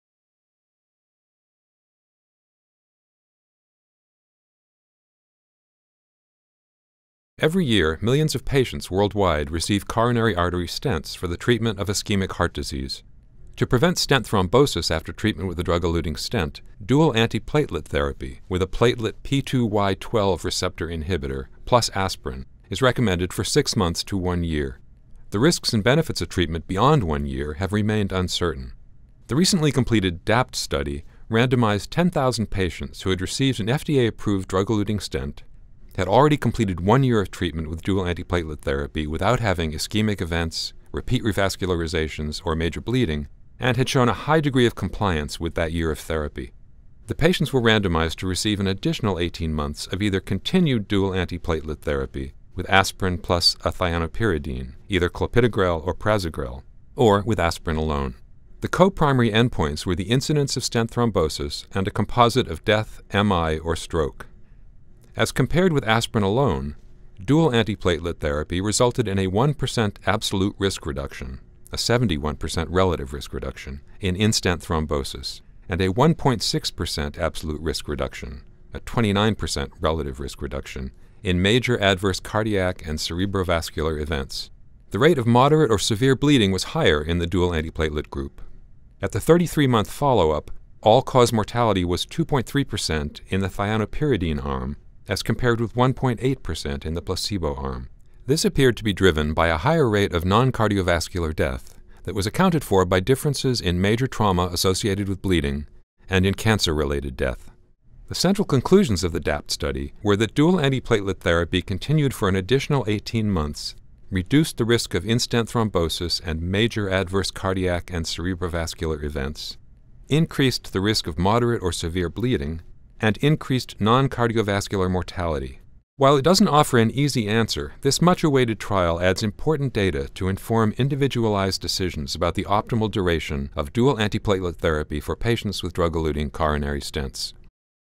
DAPT: A Concise Summary.
Summary of the DAPT trial, which found that an additional 18 months of dual antiplatelet therapy after placement of a drug-eluting stent resulted in improved cardiovascular outcomes, but more bleeding.